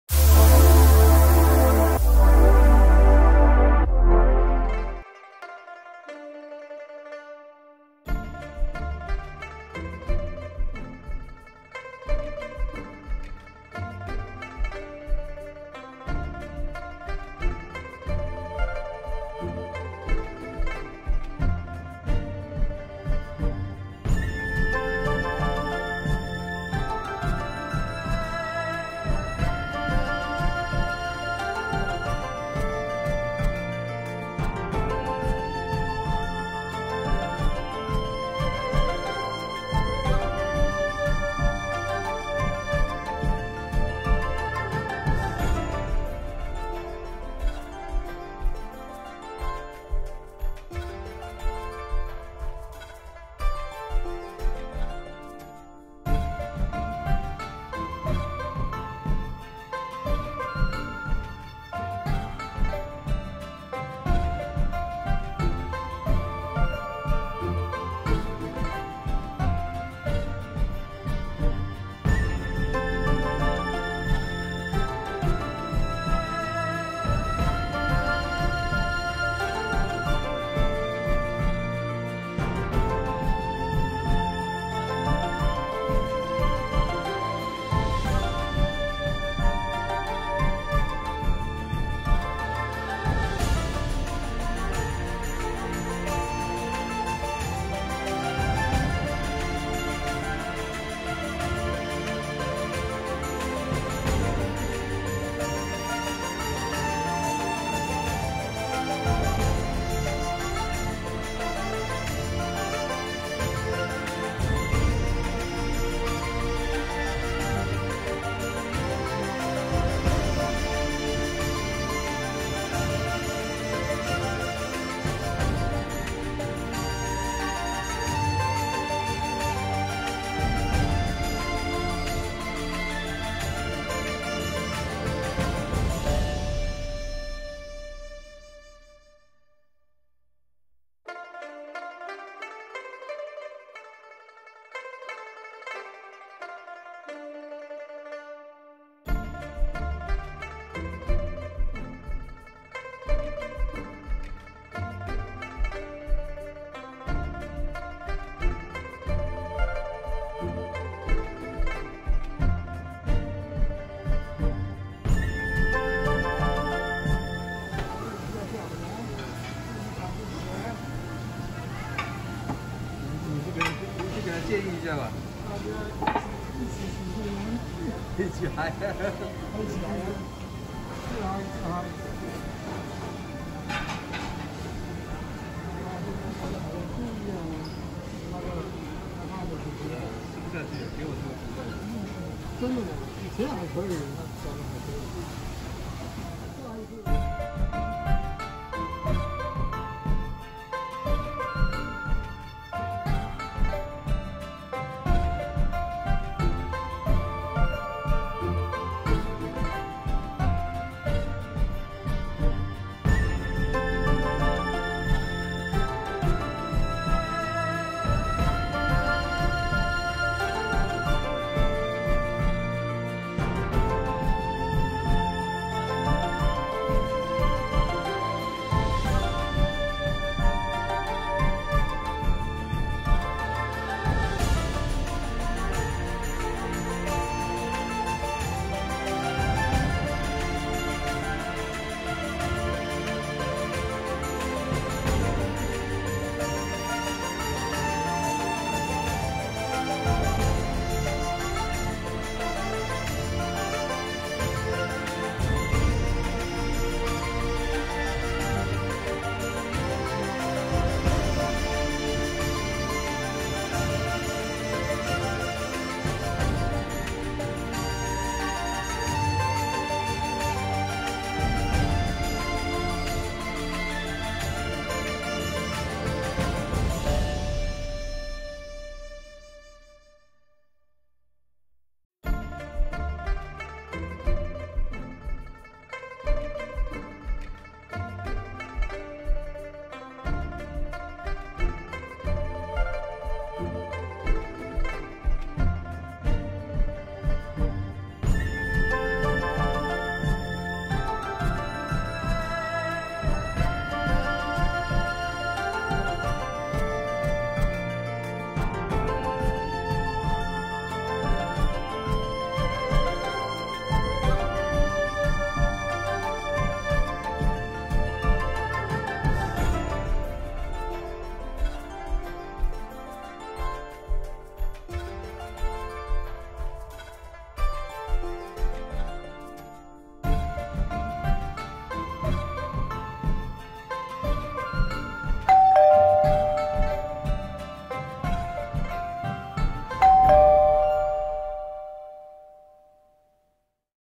KING BUFFET - HUGE SELECTION OF POPULAR CHINESE DISHES IN PLANO TX
If you’re starving and looking for something fast with many options to choose from...soups, salads, appetizers, mains , desserts...this is where you want to be! At King Buffet, you can’t go wrong! King Buffet has a wide spread of variety of dishes at a reasonable price.

We ate at their Plano restaurant; situated at E Central Parkway.